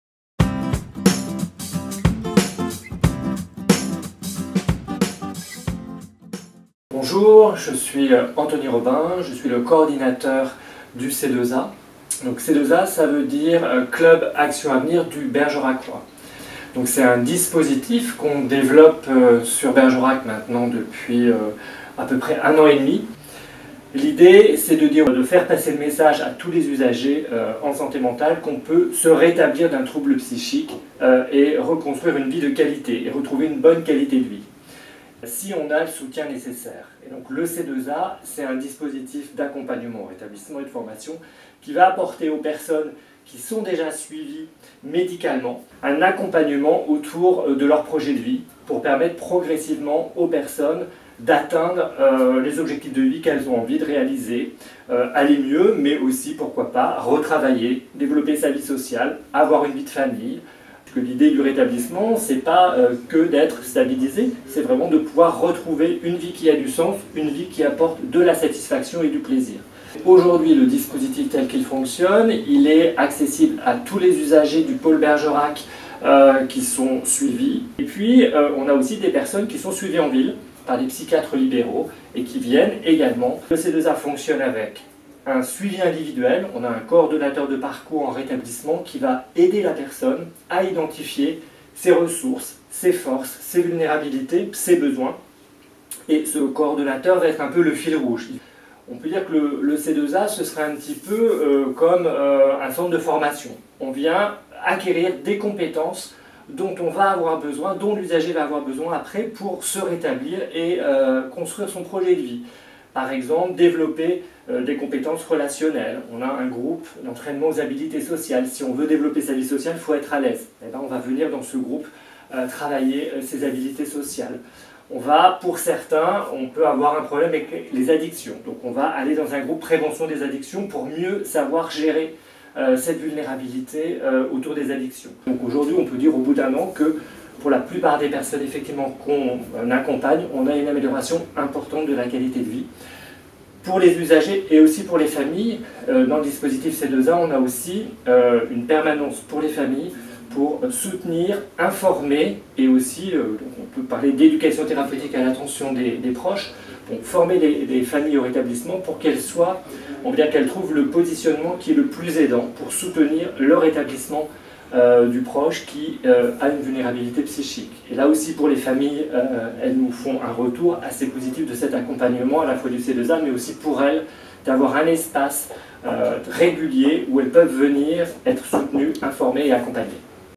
Le C2A
Dans cette courte vidéo, Antony Robin, coordinateur du C2A, Club Action Avenir du Bergeracois (Hôpital Vauclaire) nous présente ce dispositif de formation et d'accompagnement au rétablissement en Dordogne.